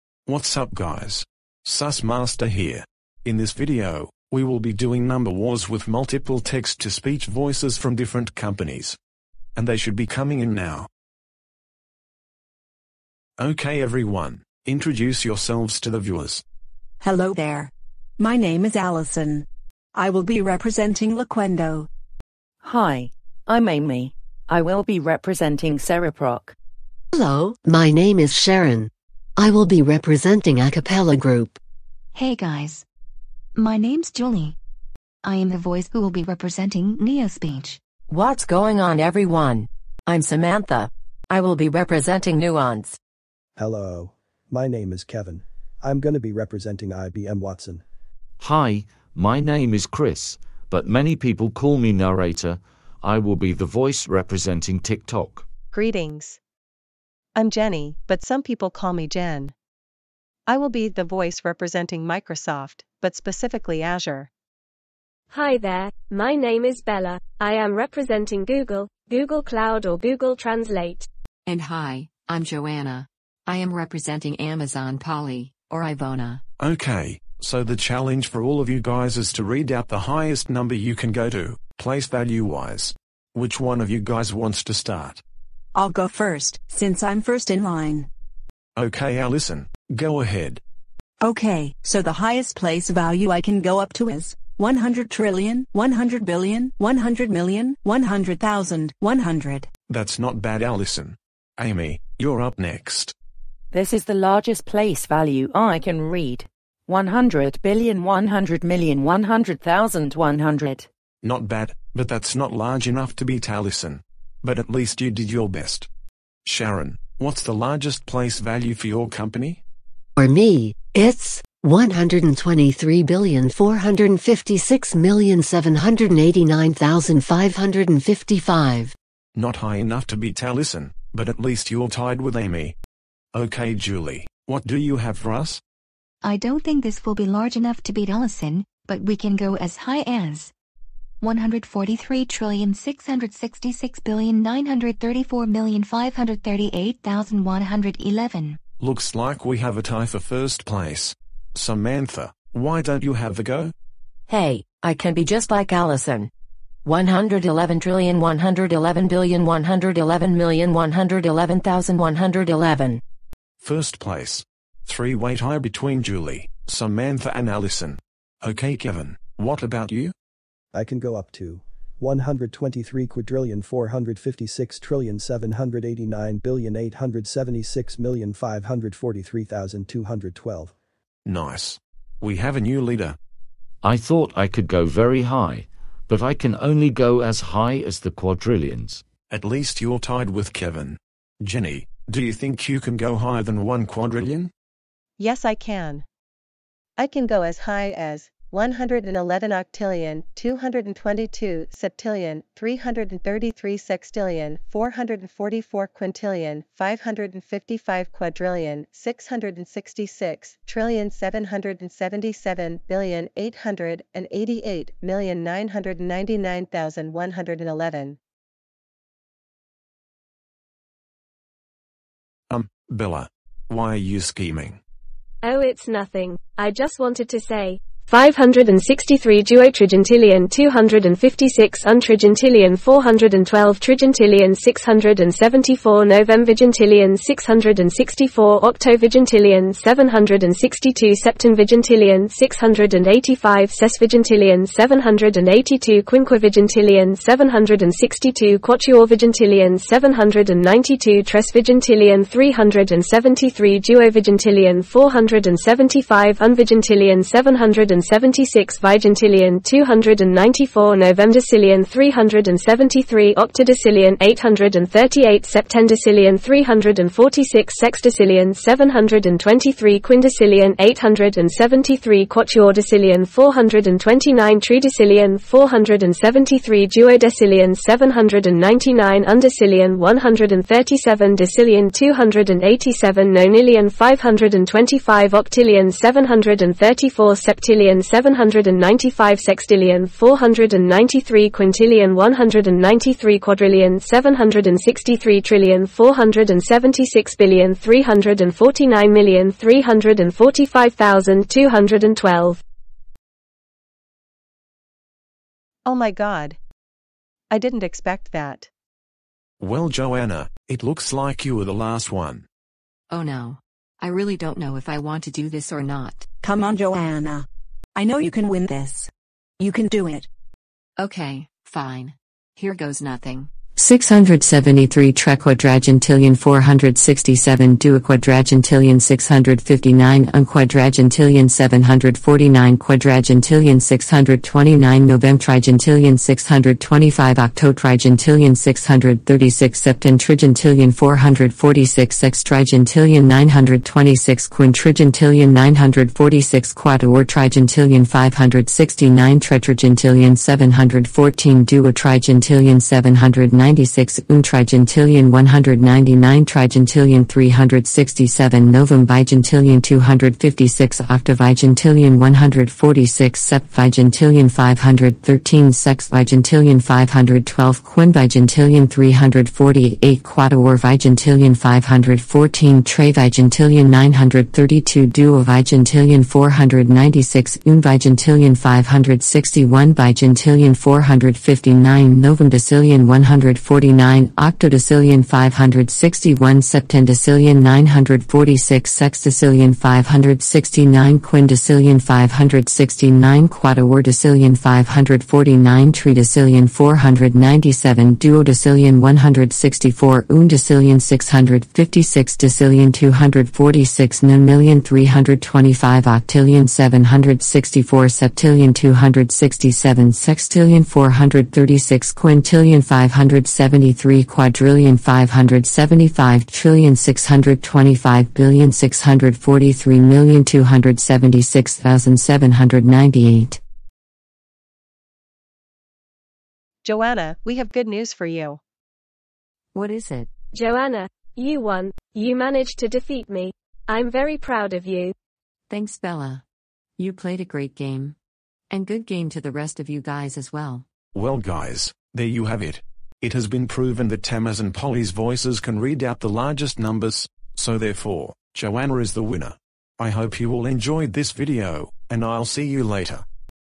Sus Master's Sussy Videos: Number wars!
I was feeling bored, so I decided to make this video.

And yes, it has been proven that Amazon Polly's voices can read out the largest numbers, all the way up to the trequadragentillions!